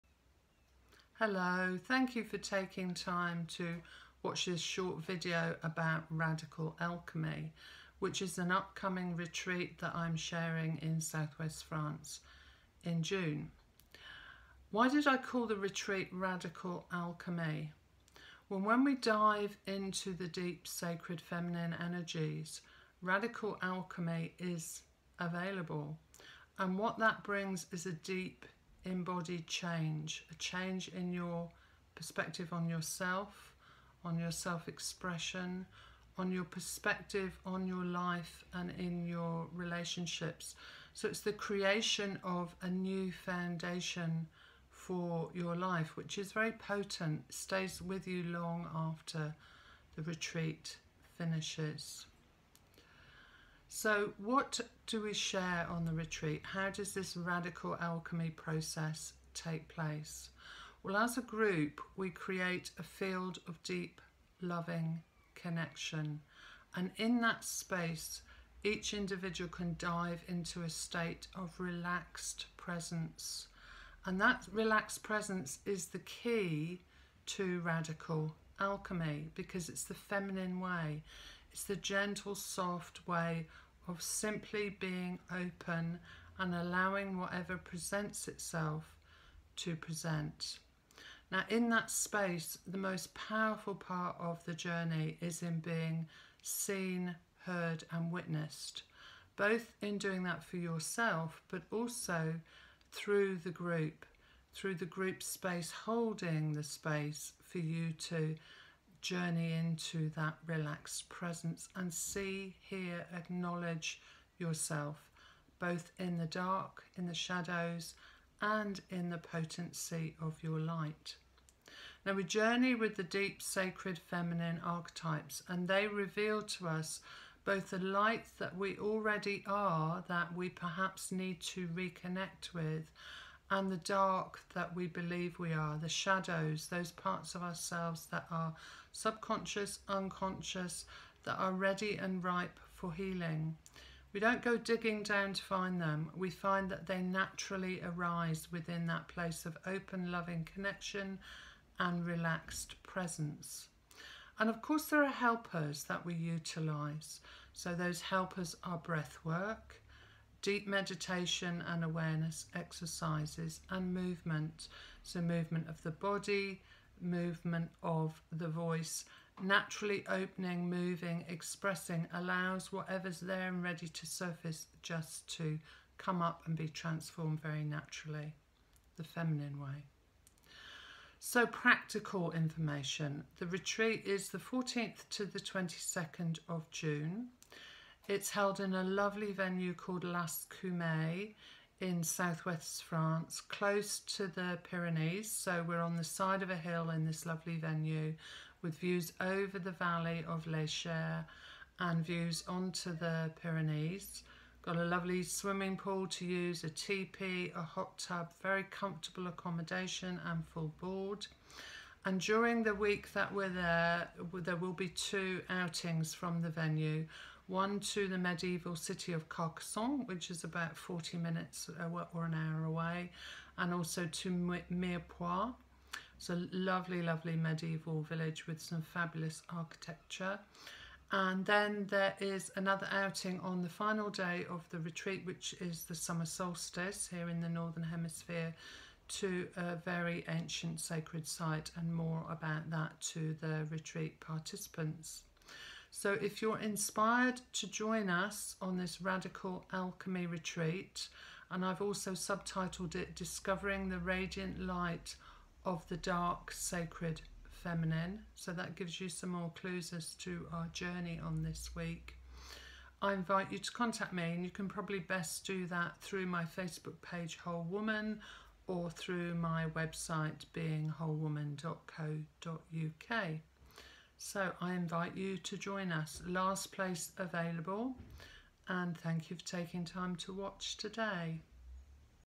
What is Radical Alchemy?
Sharing what Radical Alchemy is, what supports it and how it flows naturally through the feminine way of healing transformation. Information also about the Radical Alchemy Retreat in SW France with Whole Woman facilitator, Cassandra Eve, 14 - 22 June 2019